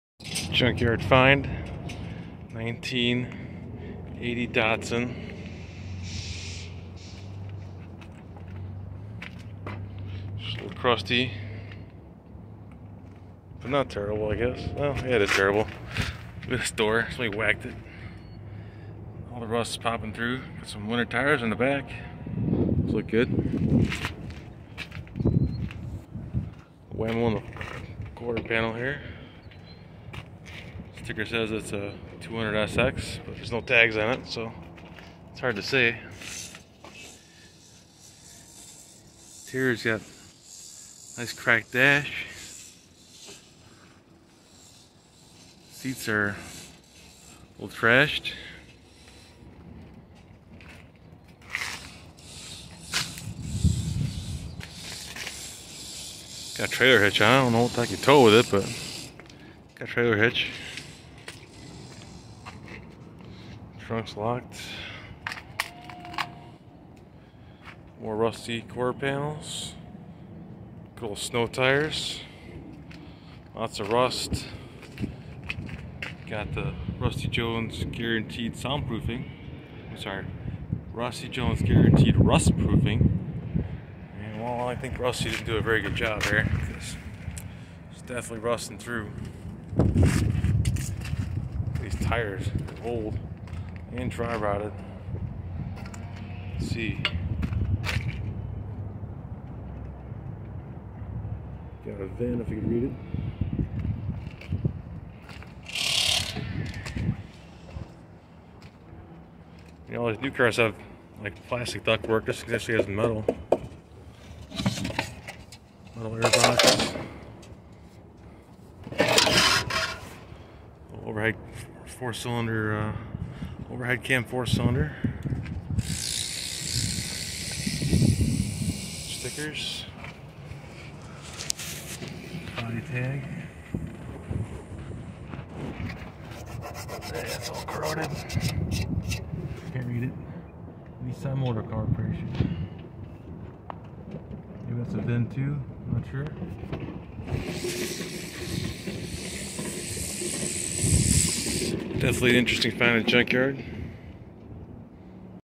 1980 Datsun 200SX junkyard find economy car 4 cylinder rusty gold
I found this 1980 Datsun 200SXin the junkyard. Is it rusty gold or a rust bucket? Even Rusty Jones couldn't save this one.